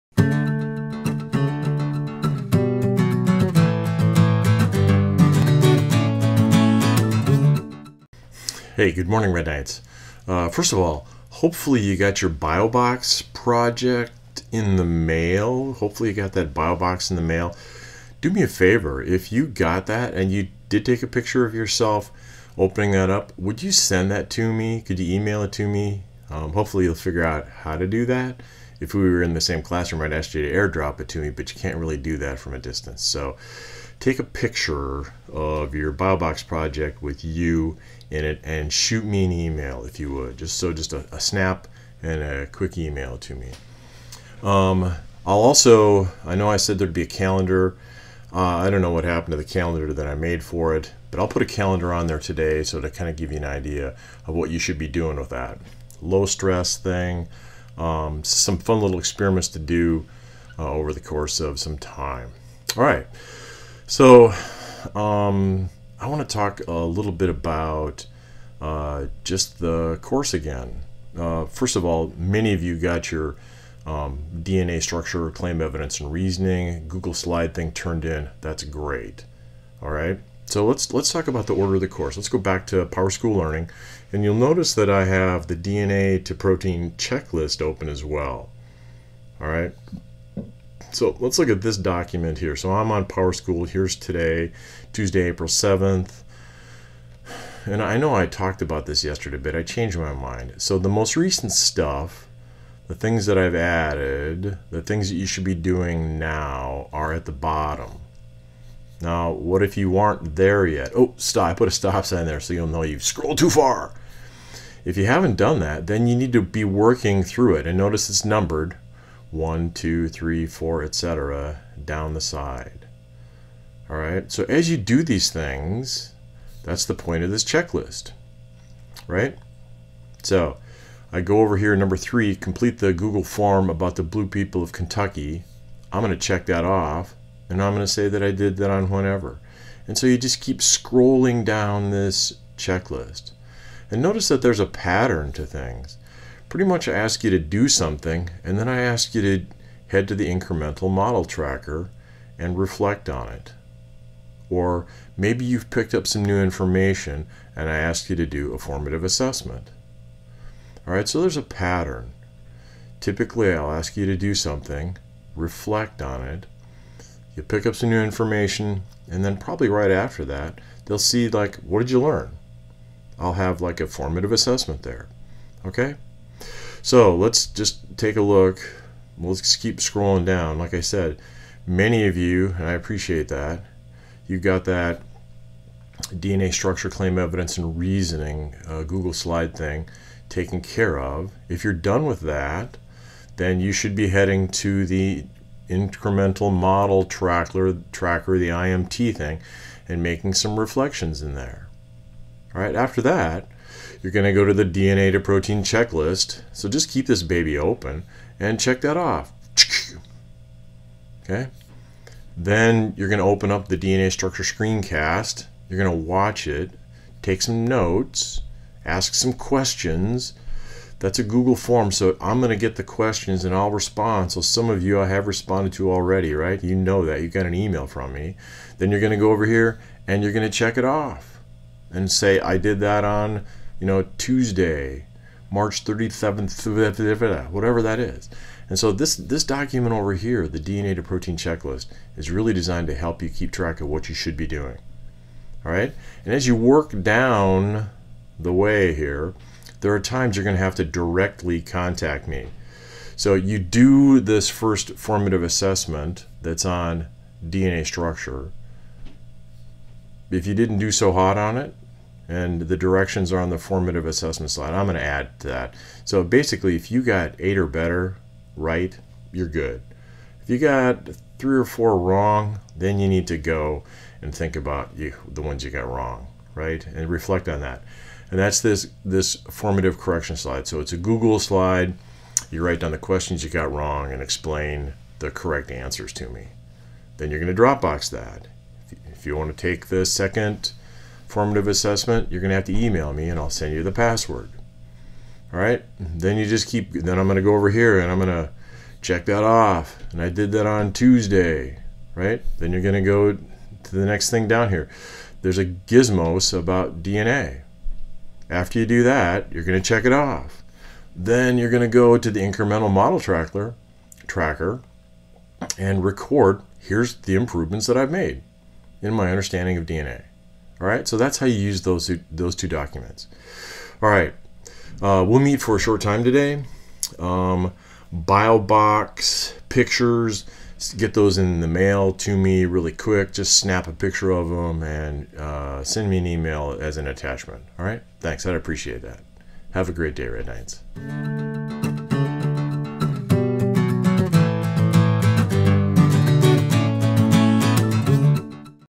Biology Tuesday April 7 2020
BioBoxProject: send me that picture of you opening up the box!
Use the DNA to Protein Checklist Spreadsheet to keep track of your workflow!